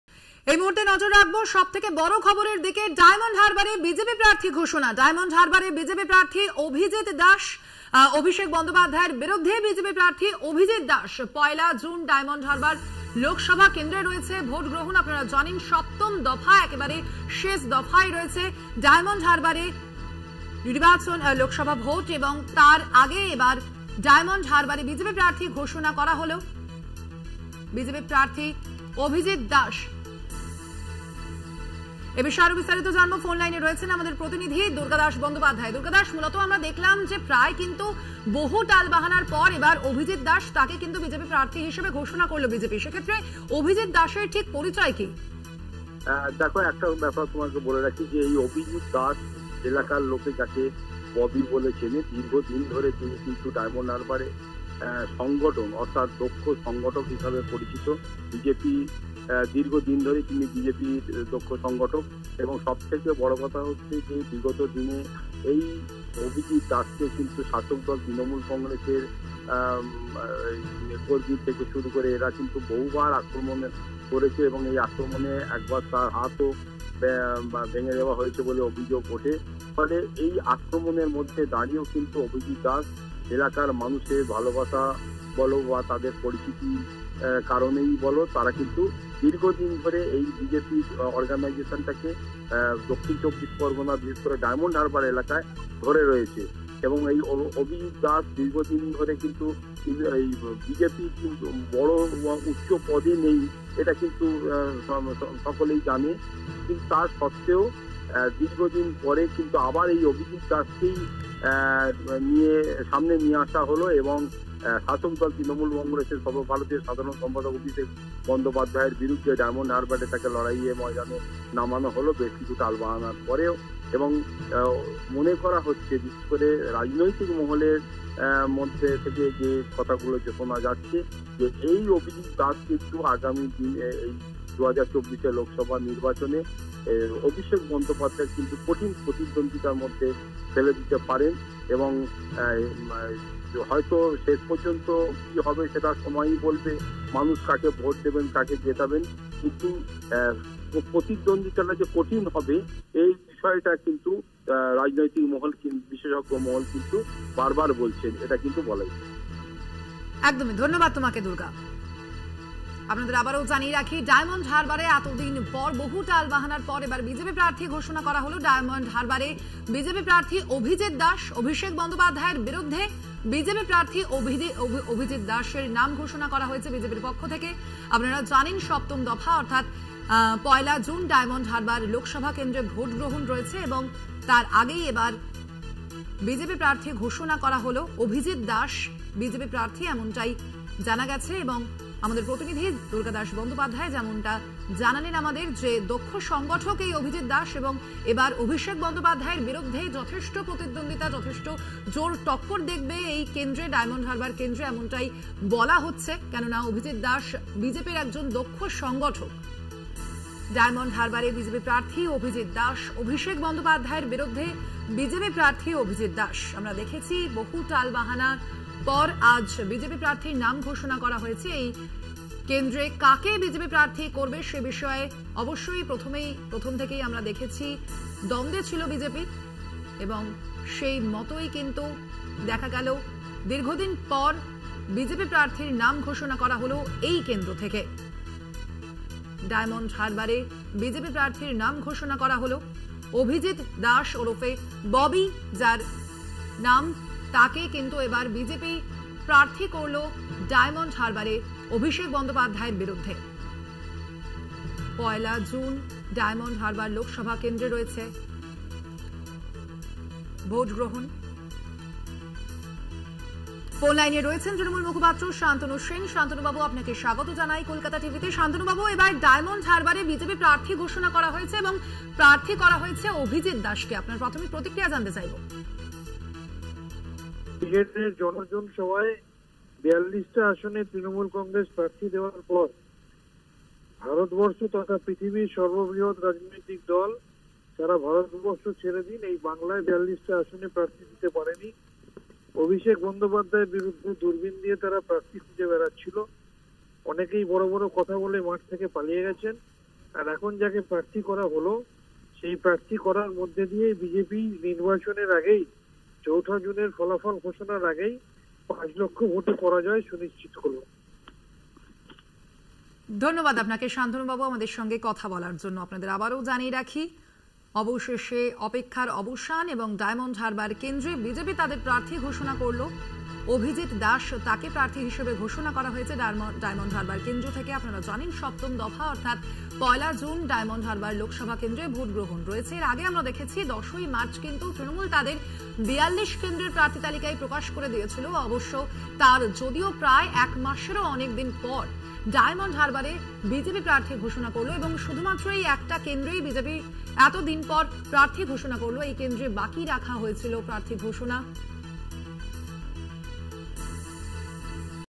Diamond Harbour BJP | অবশেষে ডায়মন্ড হারবারে বিজেপির প্রার্থী ঘোষণা, অভিষেকের বিরুদ্ধে অভিজিৎ
website link for this copy :
@Kolkata Tv